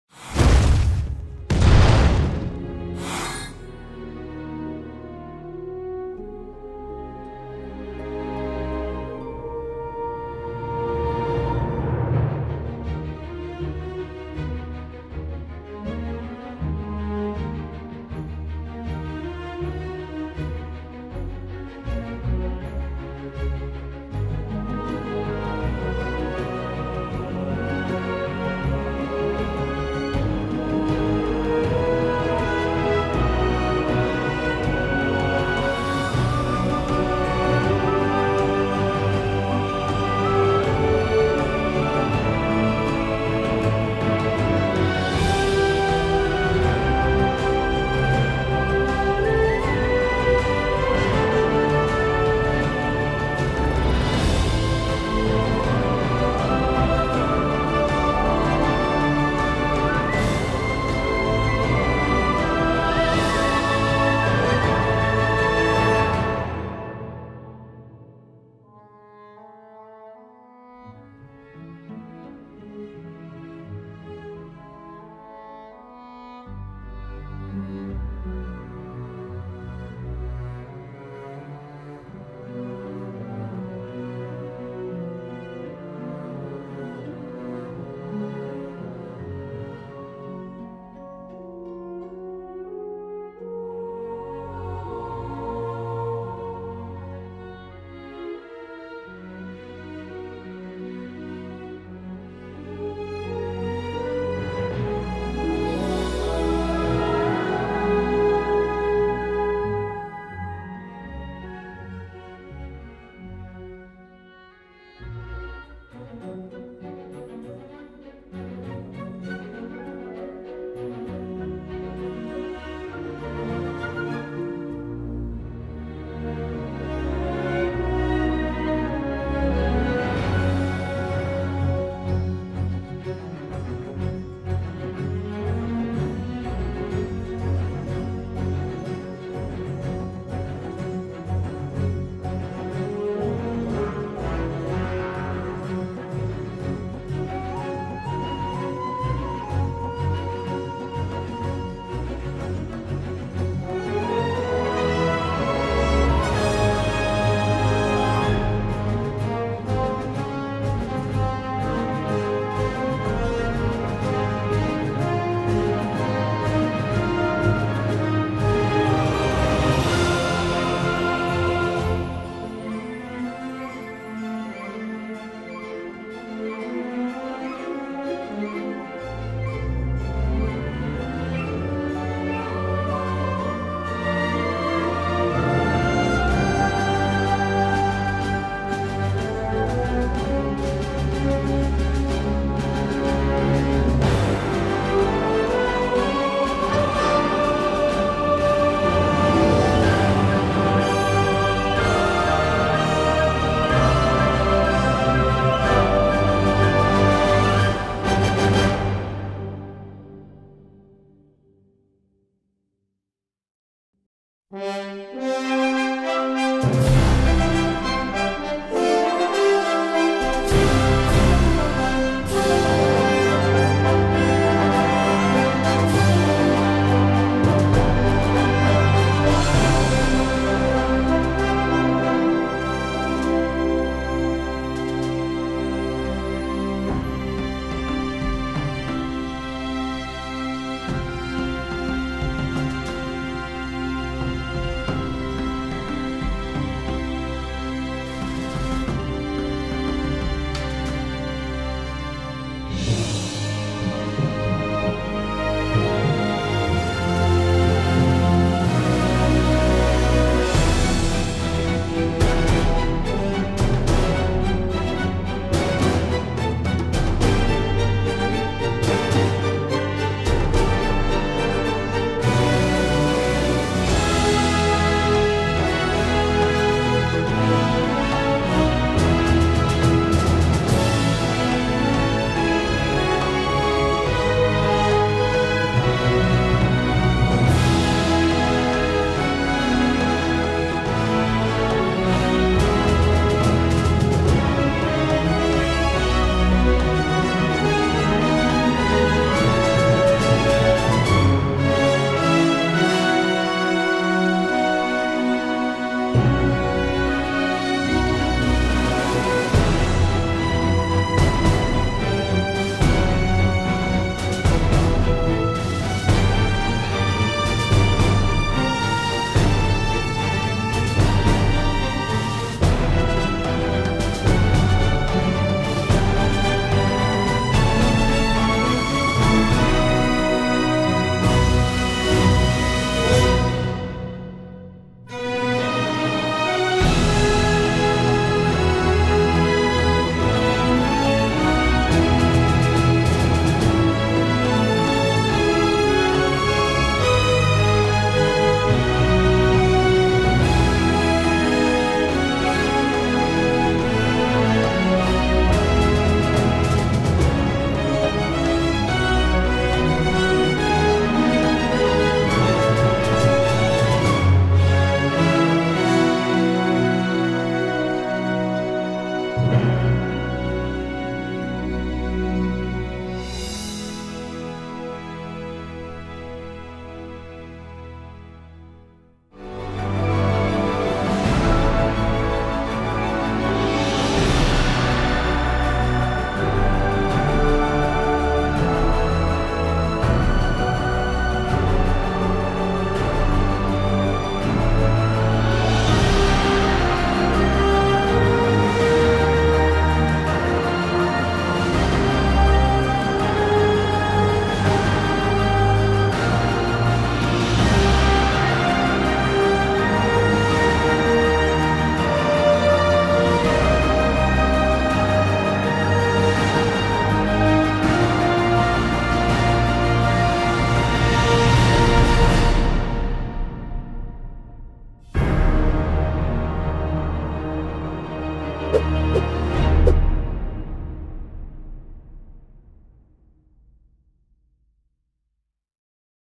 Shovel Knight Speed Paint - Creating Brawlhalla
Watch as one of our artists, Dave Montes, creates the splash art for the Shovel Knight crossover.